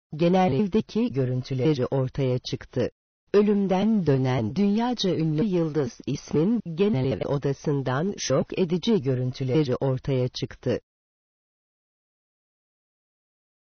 Genelevdeki görüntüleri ortaya çıktı!
Genelevdeki görüntüleri ortaya çıktı! Ölümden dönen dünyaca ünlü yıldız ismin genelev odasından şok edici görüntüleri ortaya çıktı.
Ayrıca mobil uyumlu canlı Süper Fm dinlemek için sayfasını ziyaret edebilirsiniz.